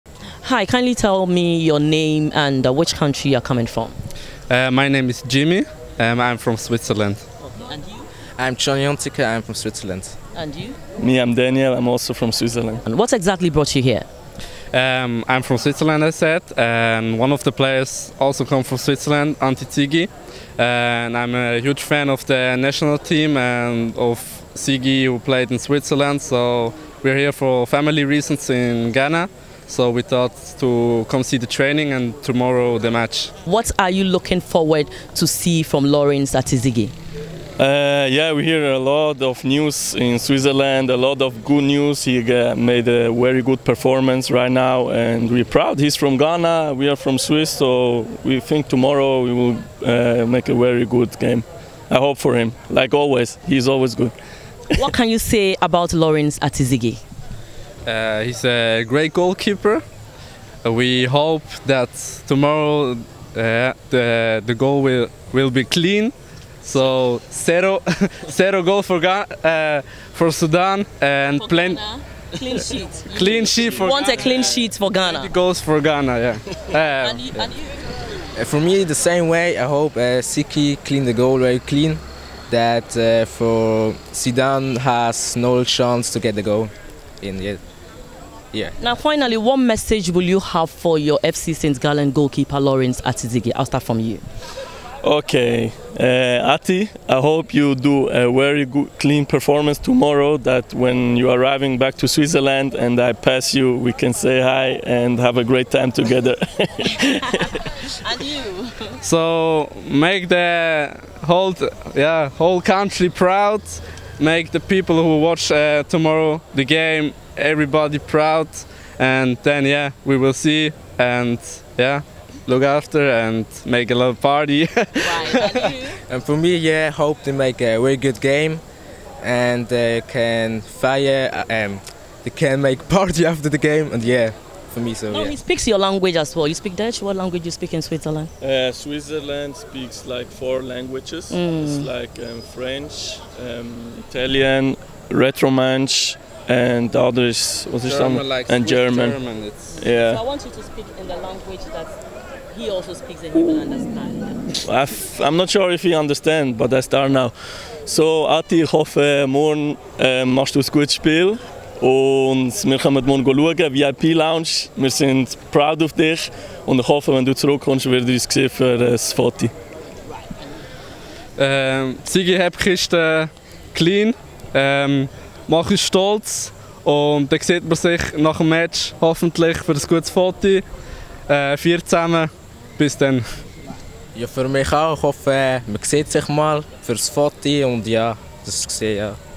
FC St. Gallen Fans Fly All The Way From Switzerland To Support The Black Stars
Some supporters of Switzerland-based FC St. Gallen have arrived in Ghana to support Black Stars goalkeeper, Lawrence Ati-Zigi in Ghana's match against Sudan tomorrow...